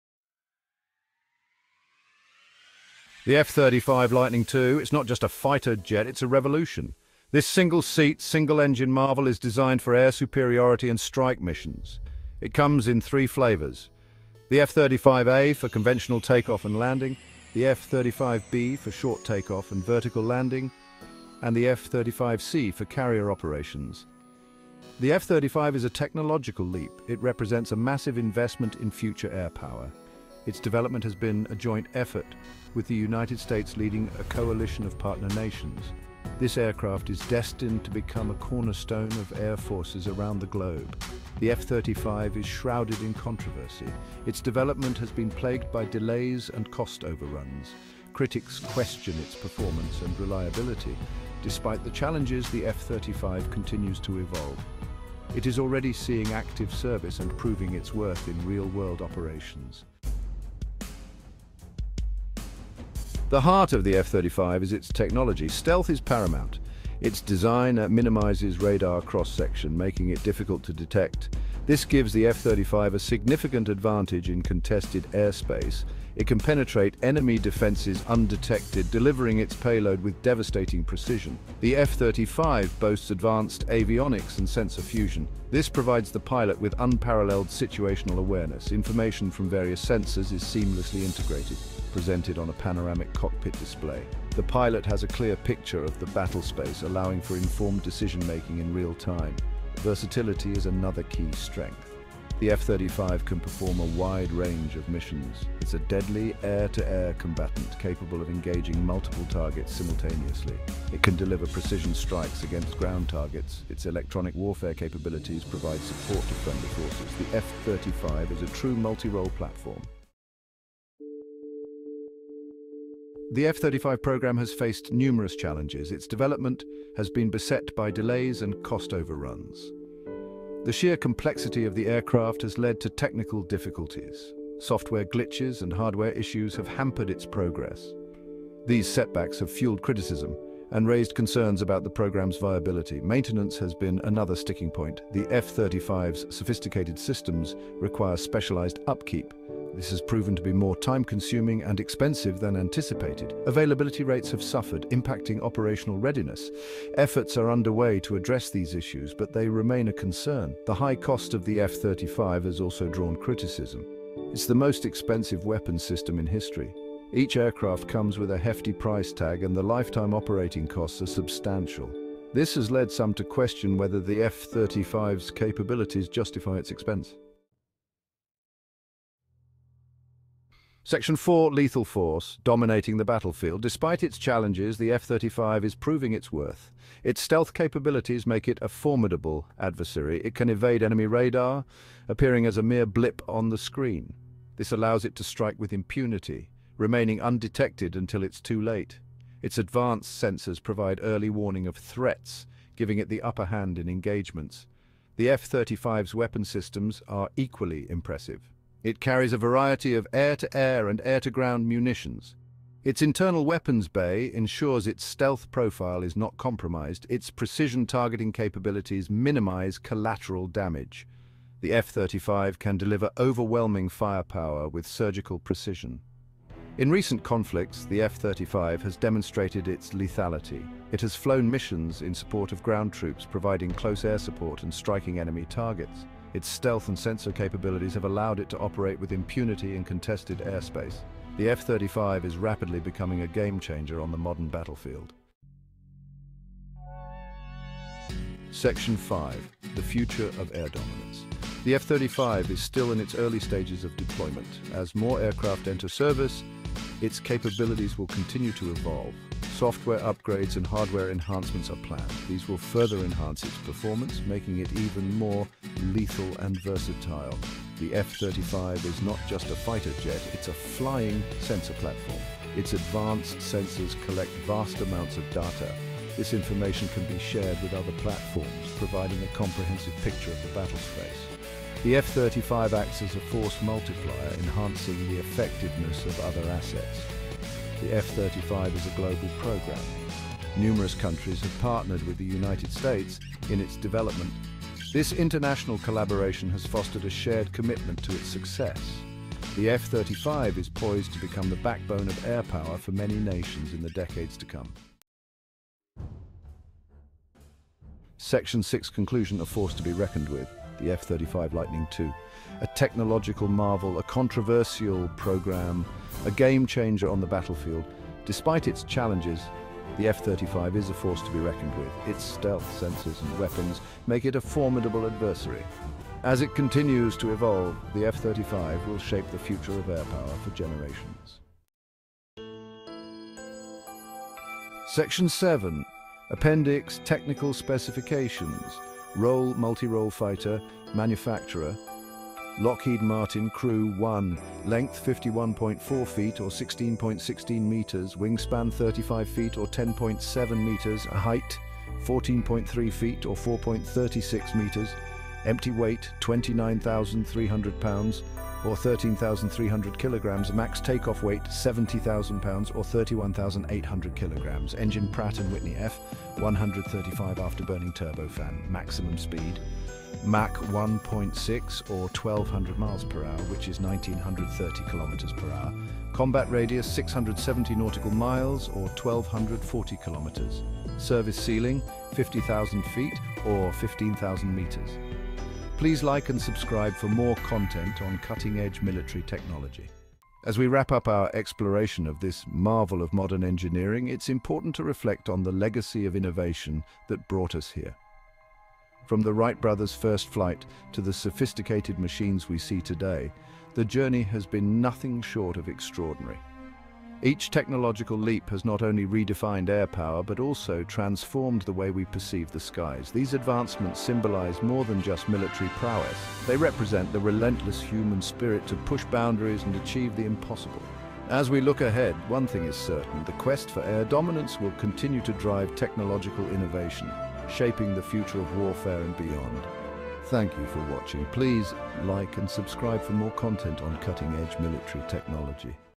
F-35 Fighter Jet: Strengths, Weaknesses, and Battlefield Lethality
Explore the world of the F-35 Lightning II in this comprehensive video! Dive into the design and capabilities of its three variants (F-35A, F-35B, F-35C) and understand why it's hailed as a significant technological marvel. We'll discuss its stealth technology, advanced avionics, and versatility in missions ranging from air-to-air combat to electronic warfare. Uncover the controversies and challenges, including development delays, costs, and maintenance issues. Learn how the F-35's evolving capabilities and international partnerships position it as a game-changer in future air power. Join us as we reflect on the evolution of aviation and the pursuit of air dominance.

OUTLINE:
00:00:00
A New Era in Air Power

00:00:59
The Cutting Edge

00:02:00
Growing Pains

00:02:57
Dominating the Battlefield

00:04:05
The Future of Air Dominance

00:05:04
A Force to Be Reckoned With

00:05:31
Technical Specifications

00:06:30
The Legacy of Innovation